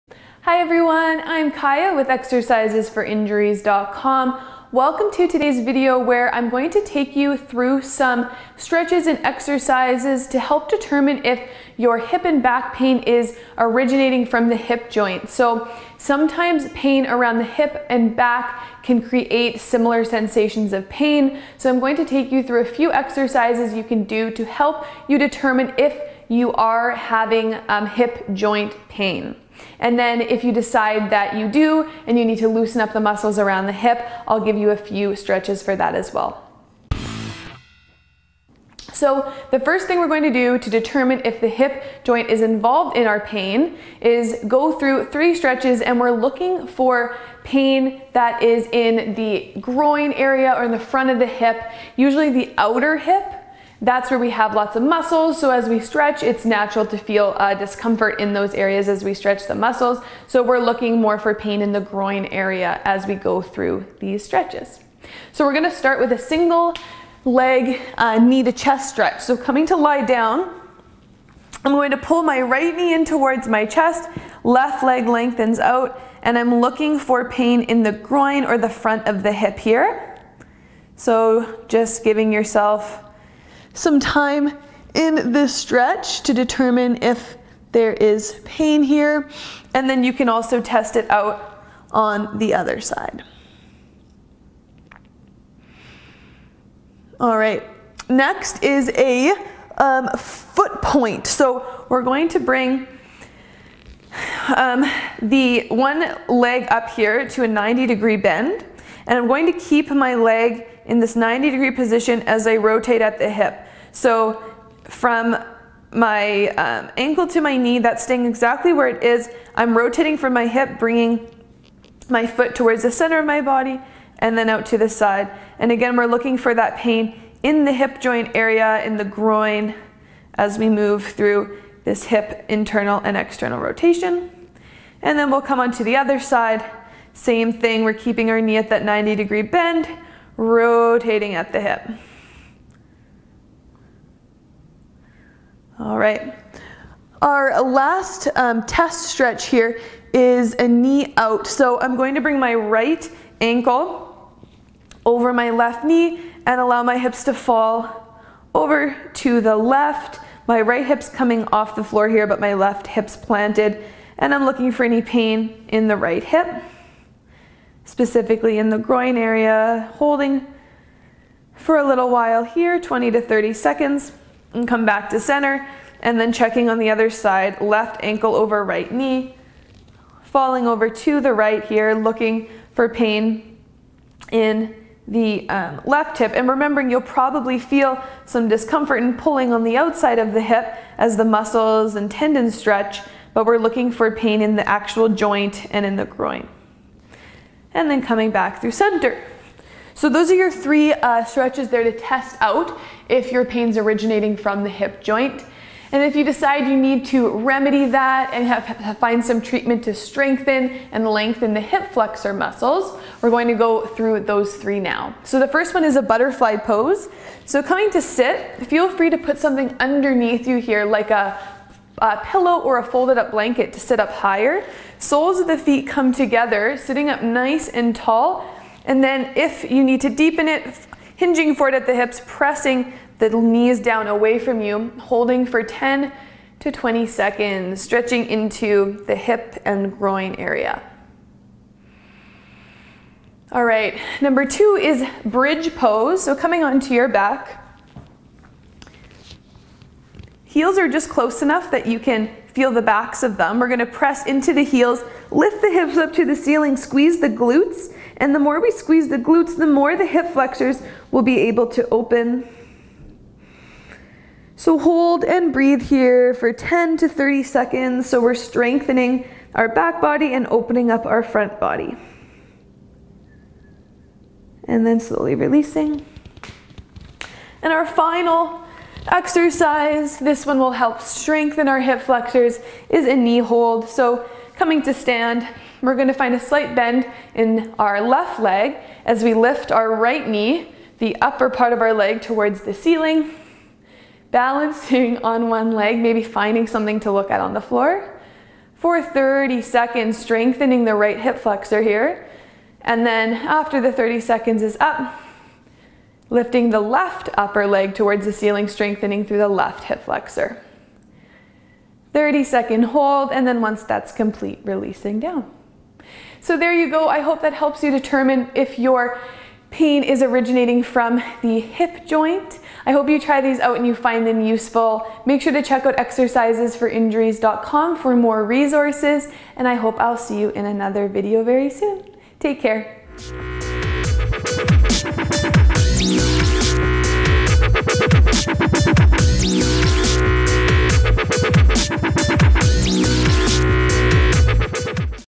Is it Hip Pain or Back Pain?
It can be difficult to distinguish between the two, but there are some clues that will help you figure out which it is. In this video, I share different ways to tell if what's ailing you is actually just Hip Pain or Back Pain!